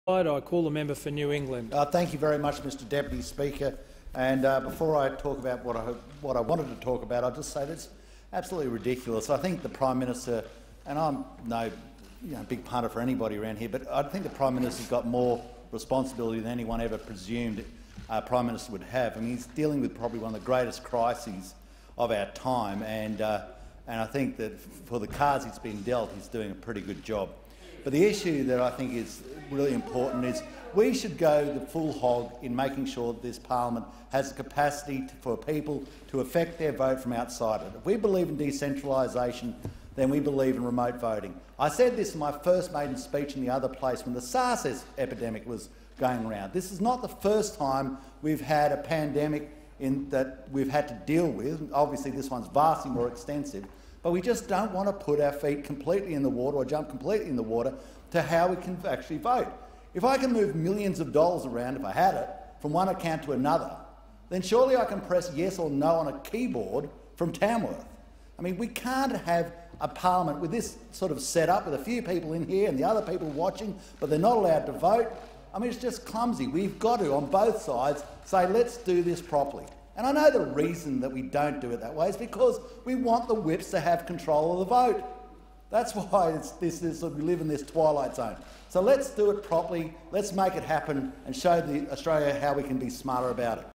Decentralisation - 24 August 2020
Speaking in the House of Representatives during 90 second statements.